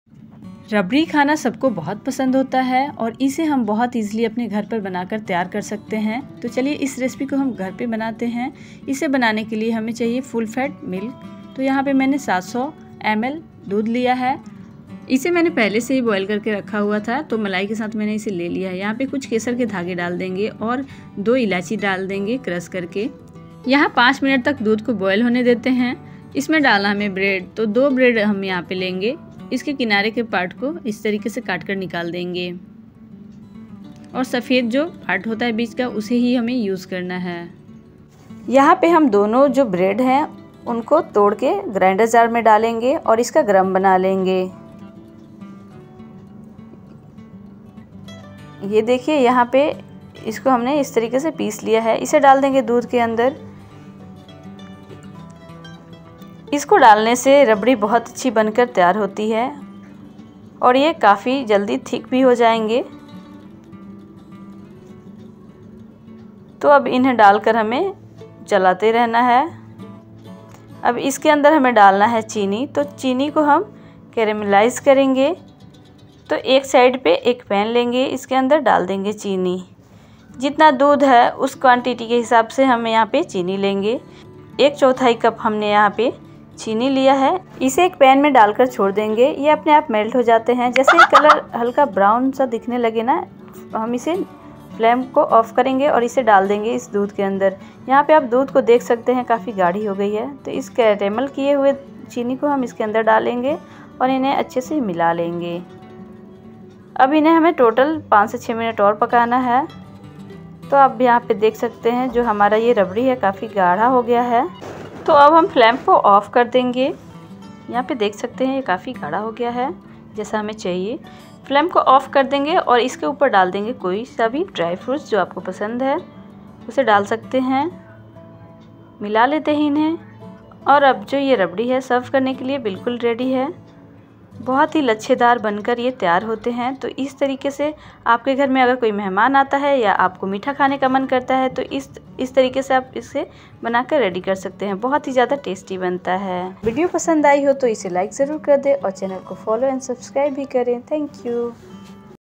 लच्छेदार रबड़ी बनाने की सरल विधि | Instant Rabri Recipe | How To Make Rabri At Home | Mom's cuisine
Hello friends! Welcome to Mom's cuisine.

Visit for other delicious recipe:-